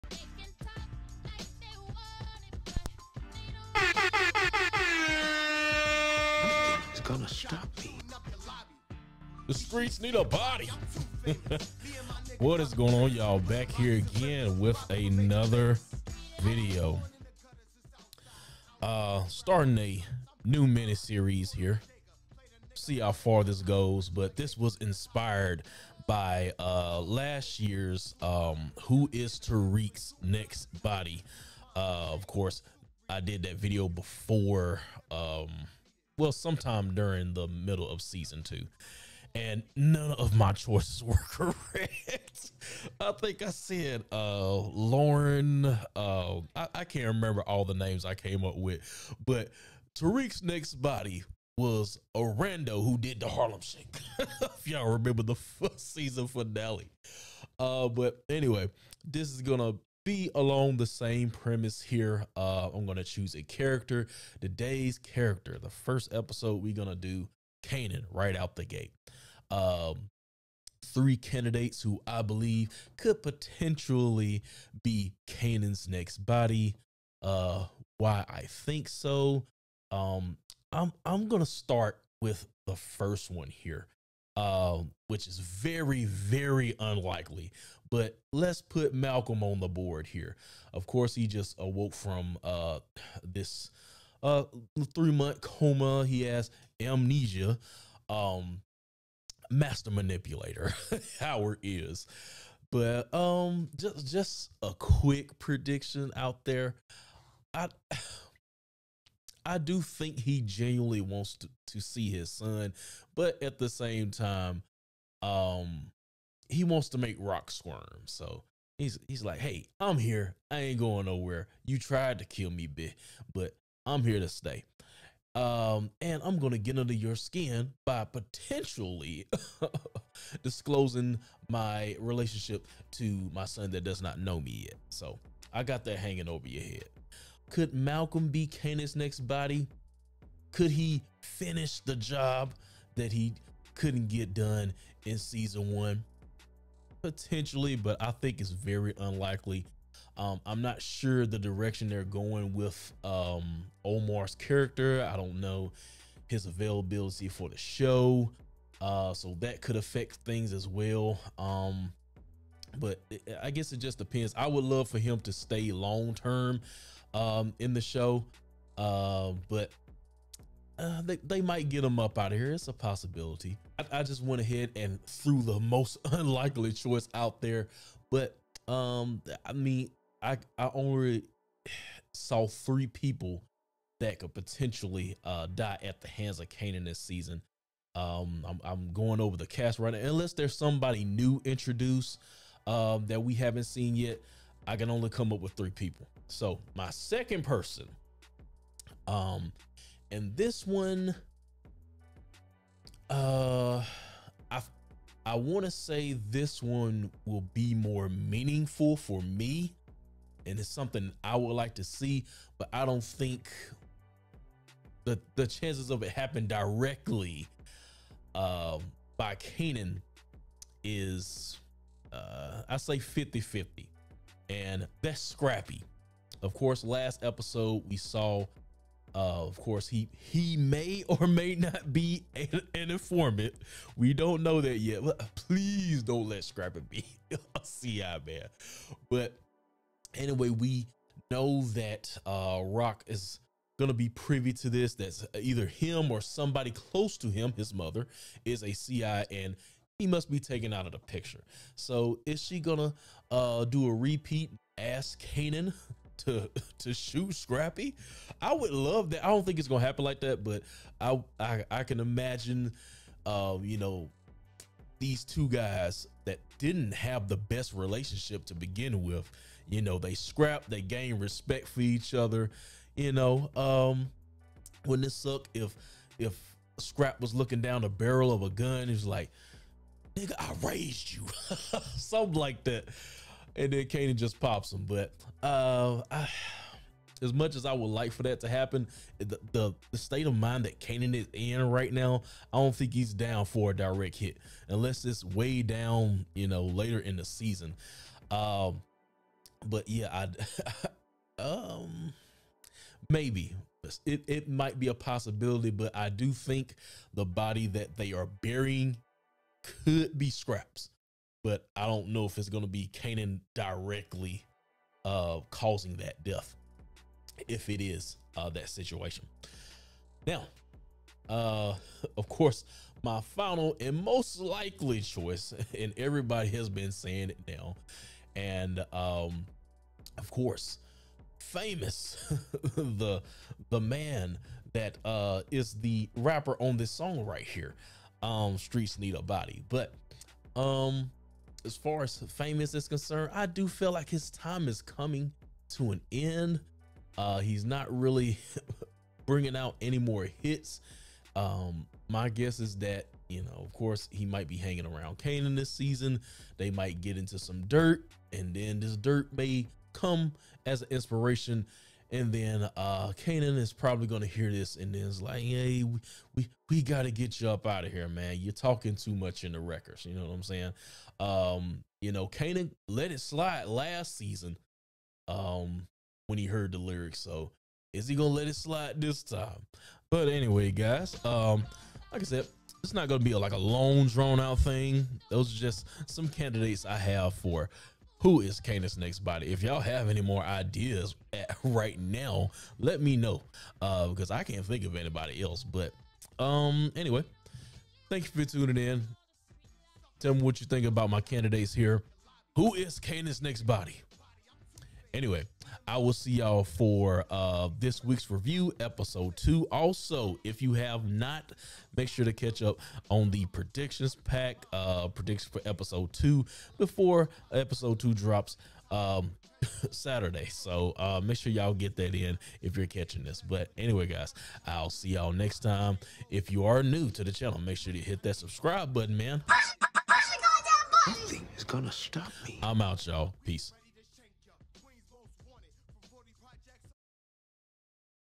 Streets Need a Body Ep.1 -Kanan’s Next Body | Raising Kanan Season 2
Streets Need a Body Episode 1

Power Book 3 Raising Kanan Season 2 Death Theories

In this debut episode I give some death predictions for season 2 of Power Book 3 Raising Kanan. In todays episode we kick it off with none other than the star of the series Kanan Stark. Who will be Kanan’s next body?